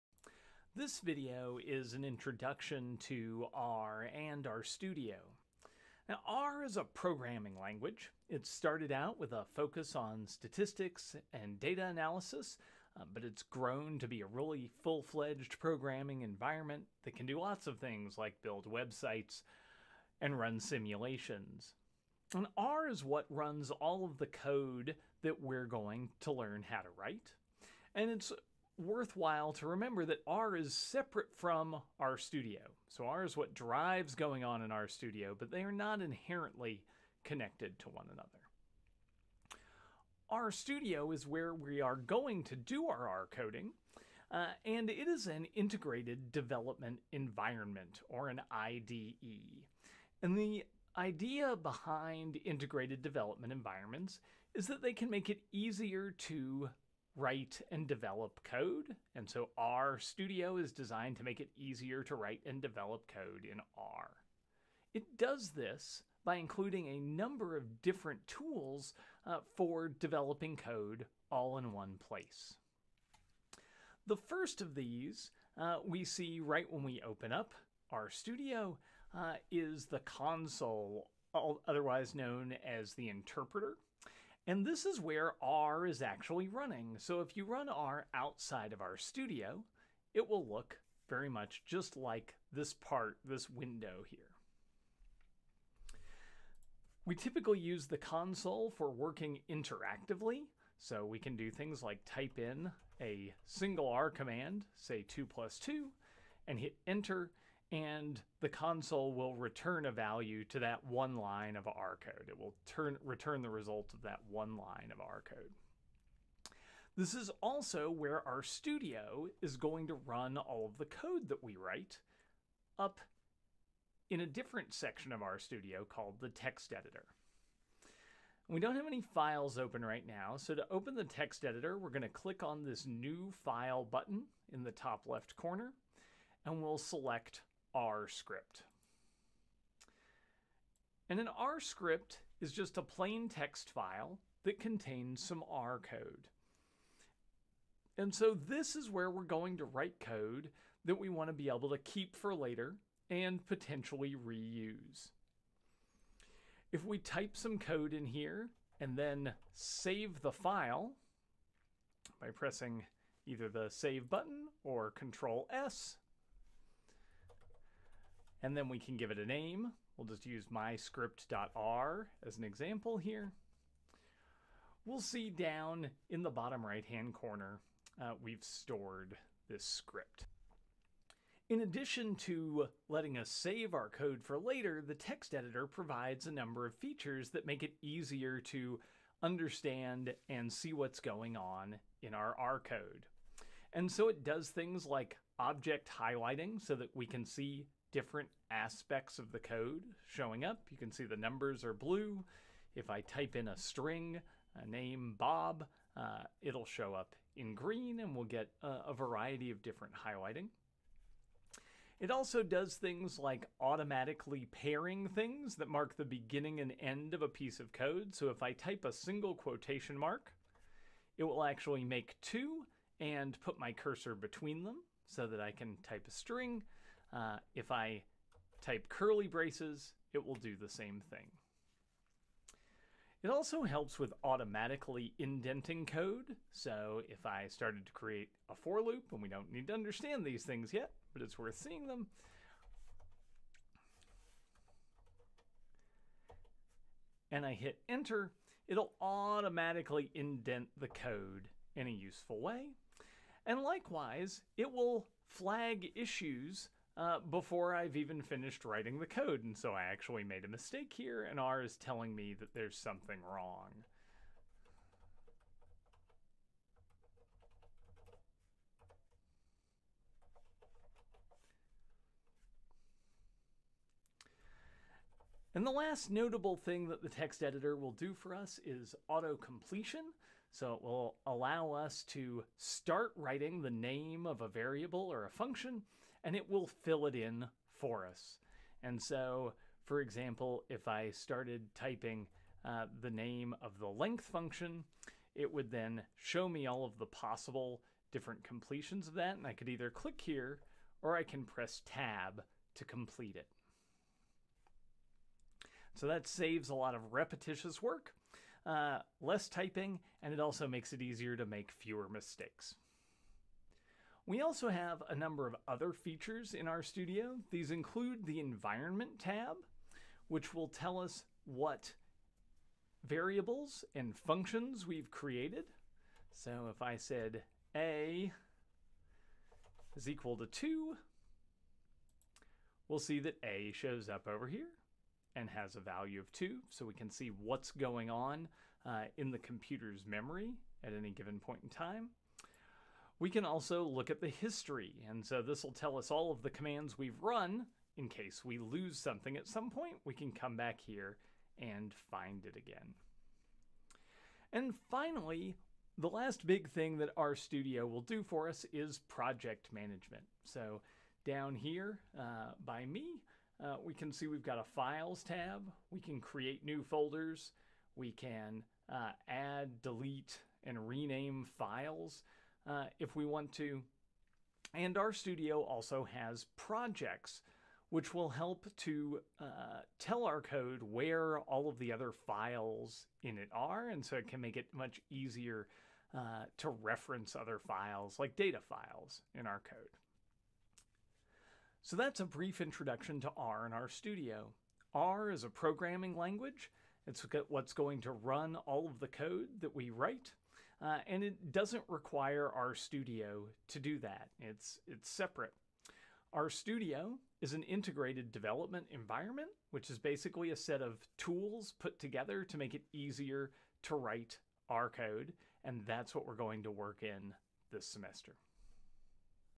Introduction to R and RStudio
A brief introduction to R and RStudio including what they are and the different features of RStudio that help us write and develop R code.